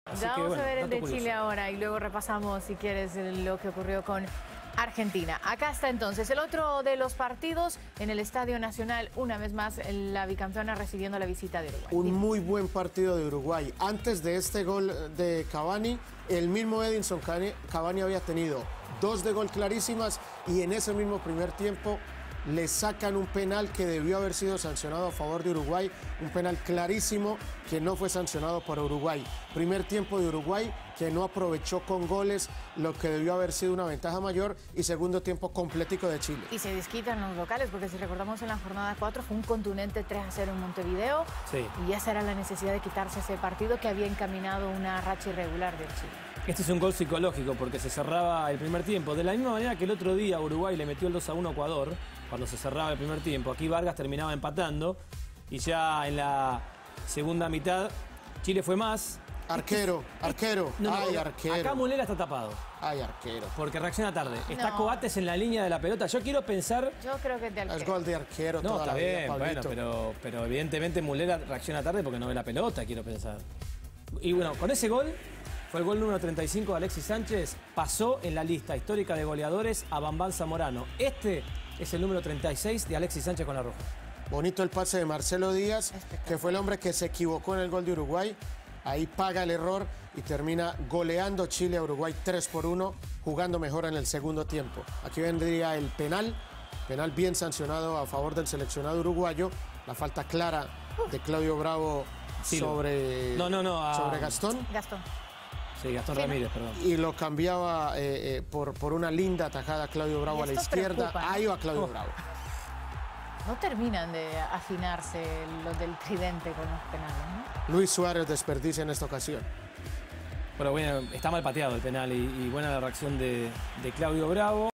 Chile 3-1 Uruguay | CONMEBOL Resumen y Goles
La Roja no dio lugar a Uruguay que sucumbió ante el poderío del campeón de América.